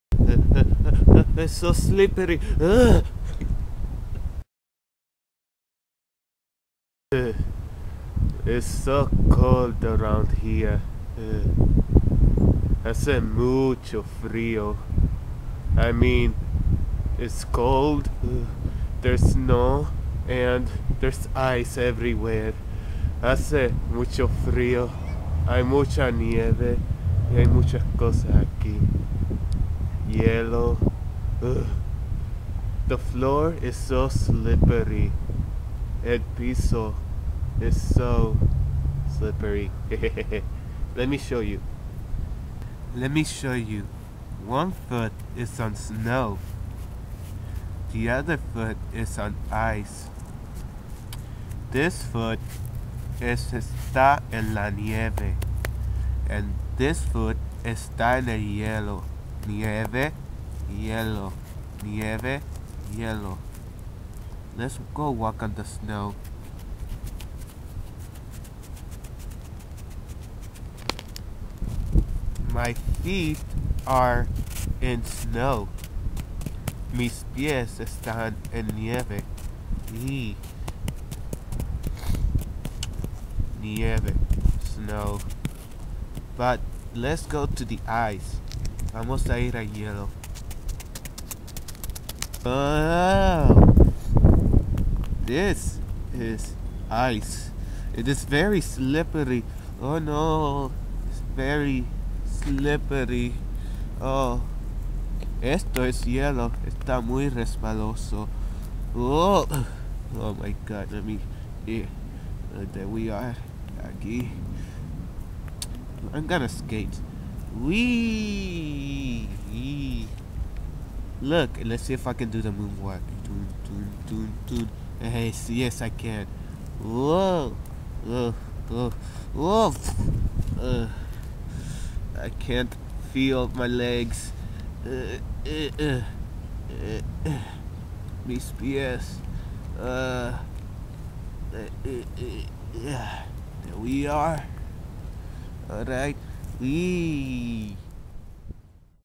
Croaker on ICE! -Texas
It's cold outside and Croaker wanted to see the snow! Yup, its snows in Texas. I'm glad Croaker din't get cold feet while filmming the video. My twitter My 2nd channel My vlogs